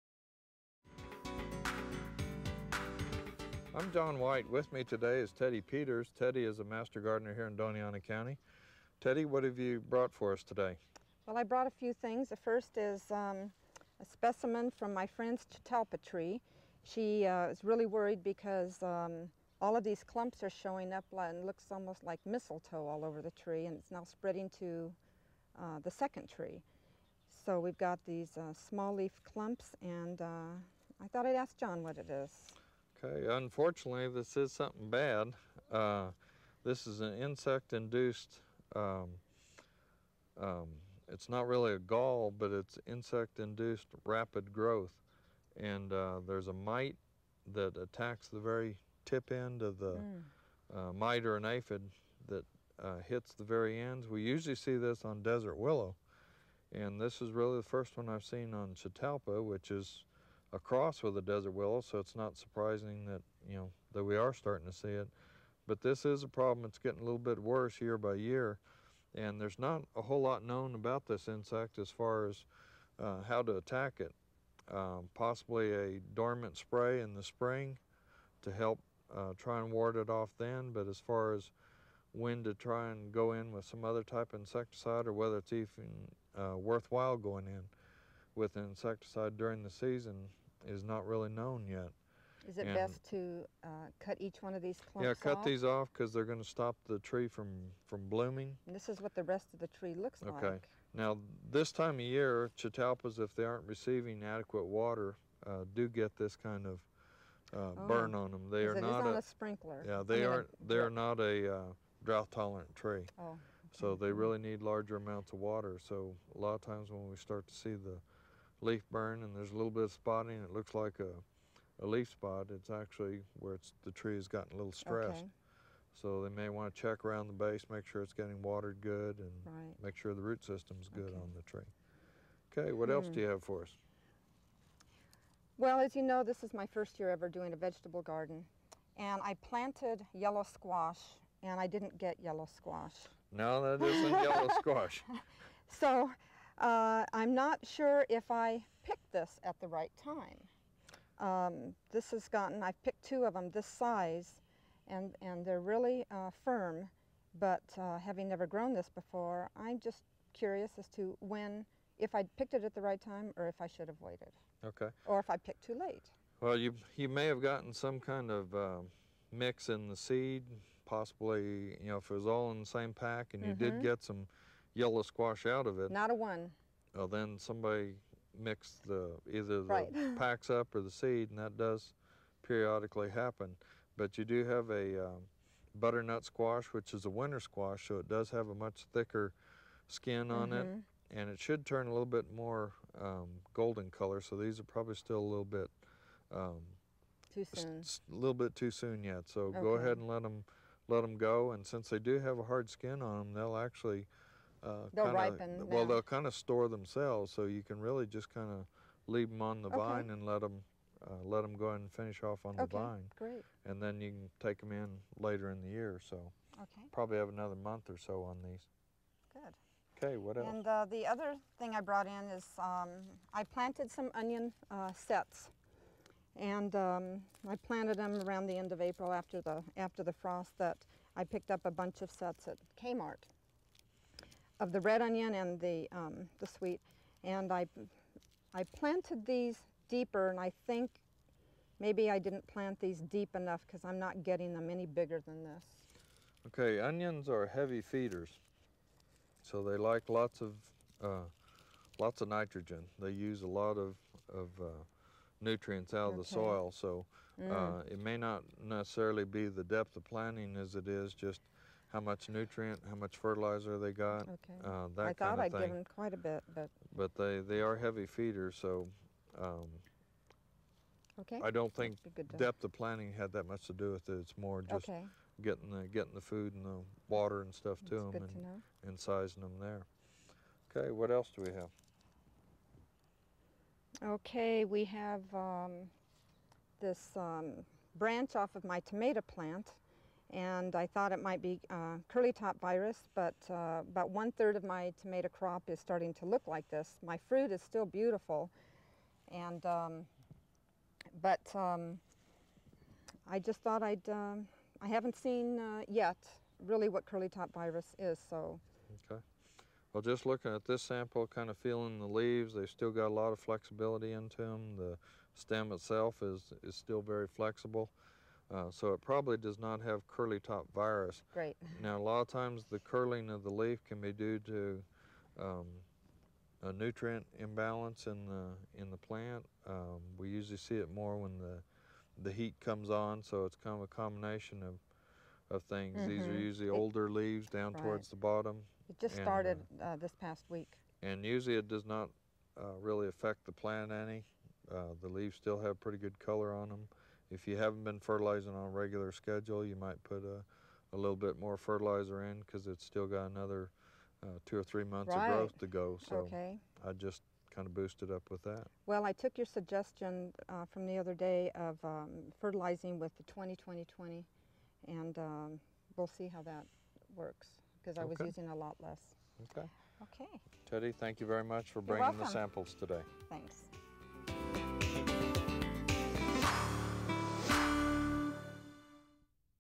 How To Identify Problems With Trees & Vegetable Plants
Learn how to identify problems with Chitalpa, Butternut Squash, Onions & Tomato plant.